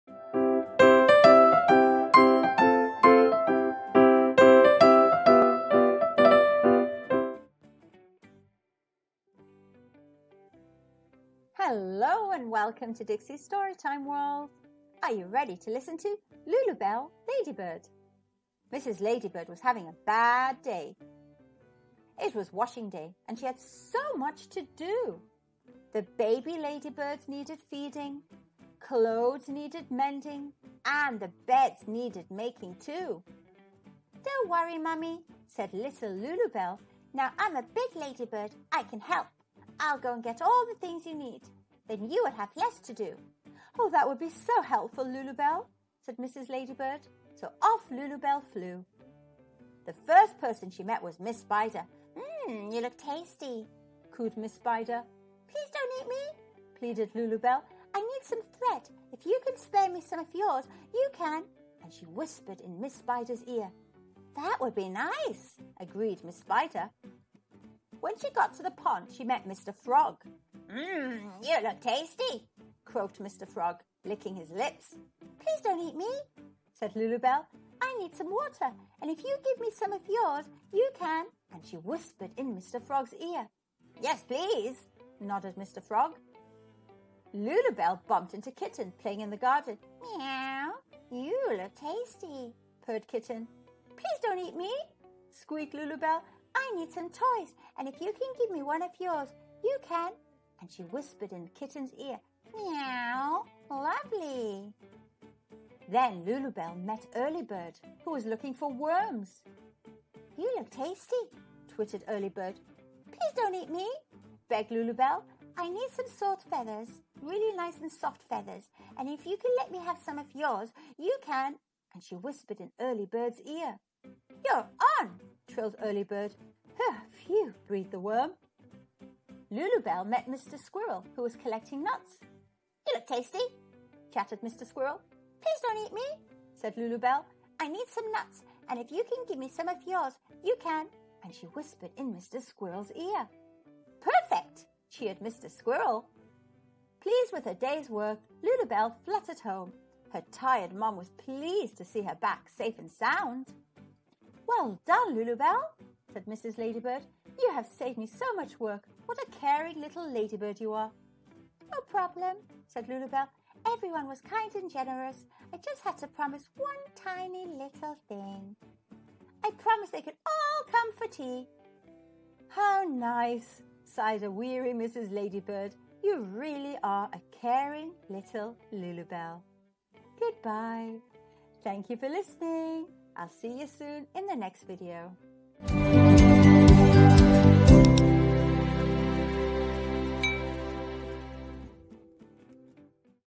💛📕Kids Books Read Aloud: Lulubell Ladybird
Mrs Ladybird was very busy, with so many children and so much to do. Thank goodness for caring Lulubell who, as usual, steps in and helps in a way that only Lulubell can!

by Sam Walshaw